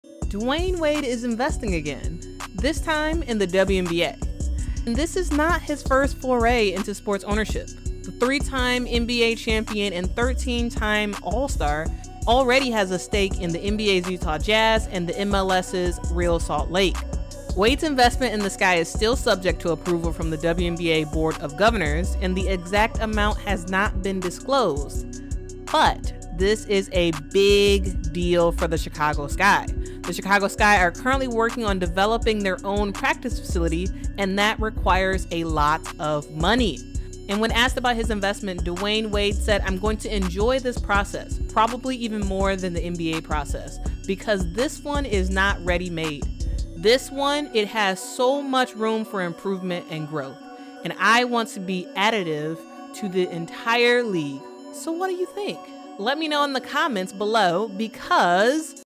Dwyane Wade Buys Ownership in WNBA’s Chicago Sky
It's official D-Wade is now officially invested in the WNBA's Chicago Sky. This is a big deal because the team is interested in acquiring a new practice facility. Let me know what you think about this new investment.